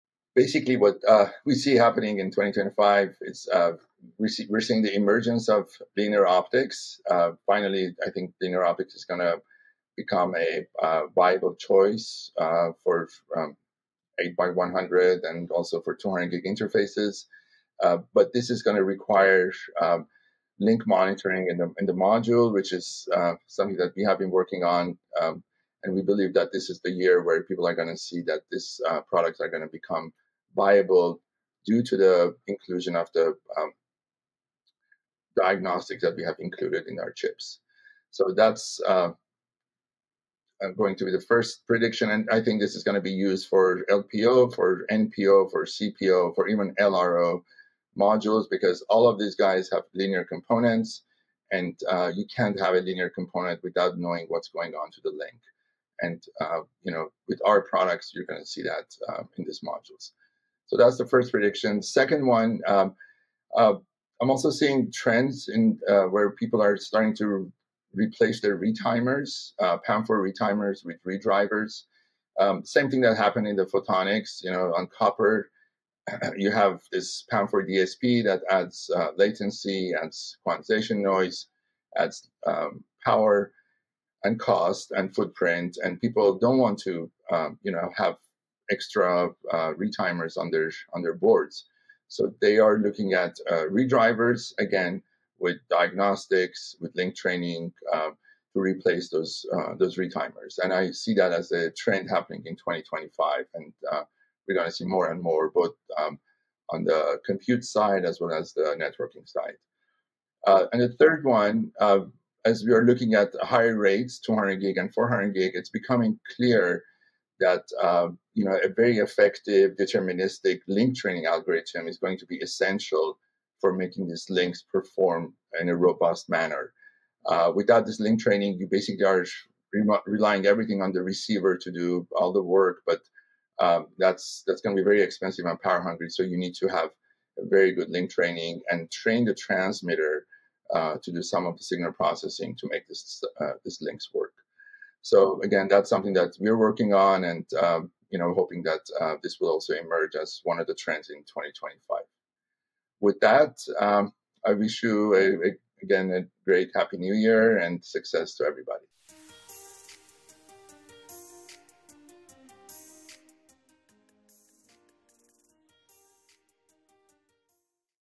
NGIPredictions2025: Linear Pluggable Optics Set to Redefine Data Centers
What's driving the evolution of optical networking in 2025?

Armond Hairapetian, Founder and CTO from TeraSignal shares three predictions:

- Linear optics emerges as the preferred choice for 8x100 and 200G interfaces, enabled by advanced link monitoring capabilities
- Retimer replacement trend accelerates as companies shift to retimer drivers to reduce latency, power consumption, and costs
- Deterministic link training becomes essential for 200G and 400G performance, optimizing transmitter-side signal processing